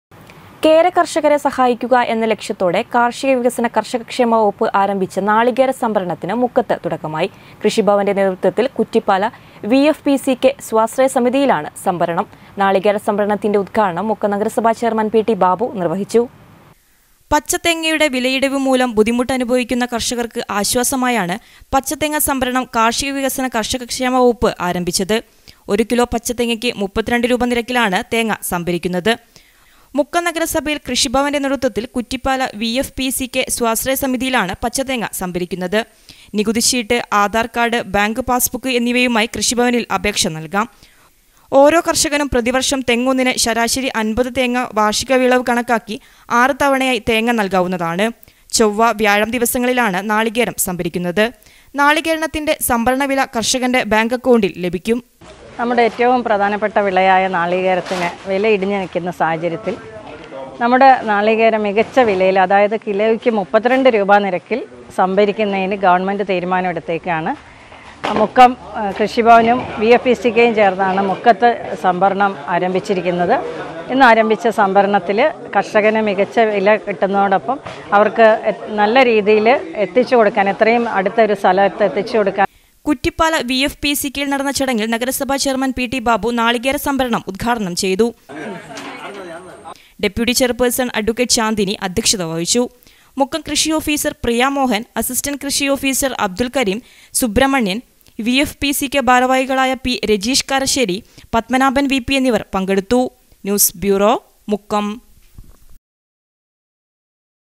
നാളികേര സംഭരണത്തിന് മുക്കത്ത് തുടക്കമായി
കേര കർഷകരെ സഹായിക്കുകഎന്ന ലക്ഷ്യത്തോടെ  കാർഷിക വികസന കർഷക ക്ഷേമ വകുപ്പ് ആരംഭിച്ച നാളികേര സംഭരണത്തിന് മുക്കത്ത് തുടക്കമായി.
 കൃഷിഭവന്റെ നേതൃത്വത്തിൽ  കുറ്റിപ്പാല വി.എഫ്.പി.സി.കെ സ്വാശ്രയ സമിതിയിലാണ് സംഭരണം.